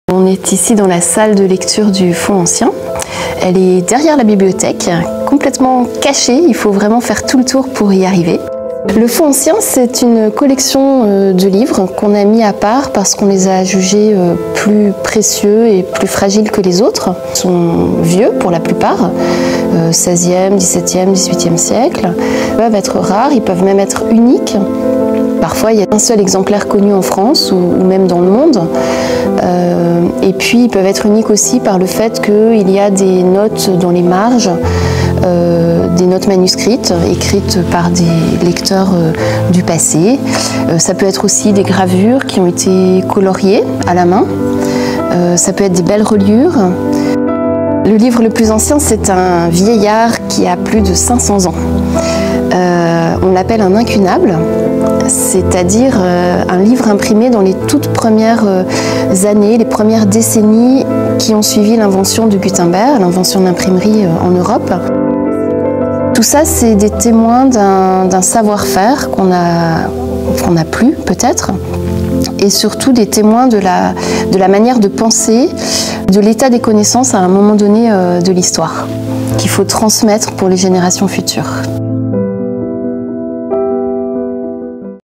Le fonds ancien de l'Université Rennes 2 : un lieu chargé d'histoire.
Le fonds ancien est une collection de 55 000 volumes précieux et fragiles qui datent d'avant la première guerre mondiale.

Le livre le plus ancien, que l'on appelle incunable, a plus de 500 ans.

Accessible à toutes et à tous du lundi au vendredi, de 14h à 17h.